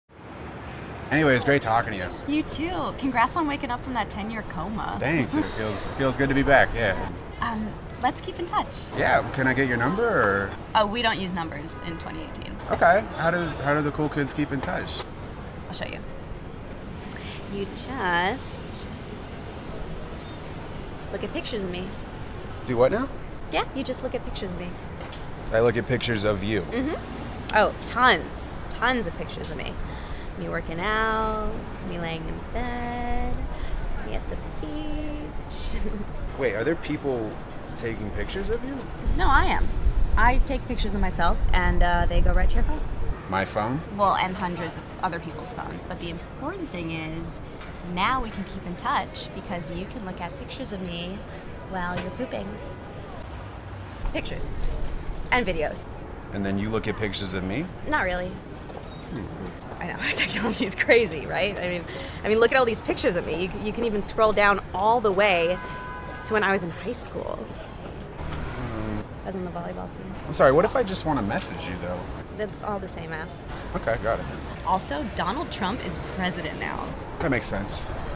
Keep In Touch
How the cool kids keep in touch in 2018. Starring Shana Goodman and Jesse Perkins.